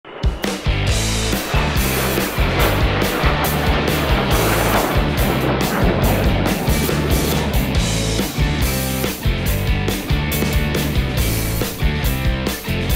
Airplane Quiz: Cold War Plane 105 Level Beginner
Aviation military history
✈ FLYING QUIZ game  👉 Guess the right aircraft out of 3 potential answers
✈ F/A-18 Hornet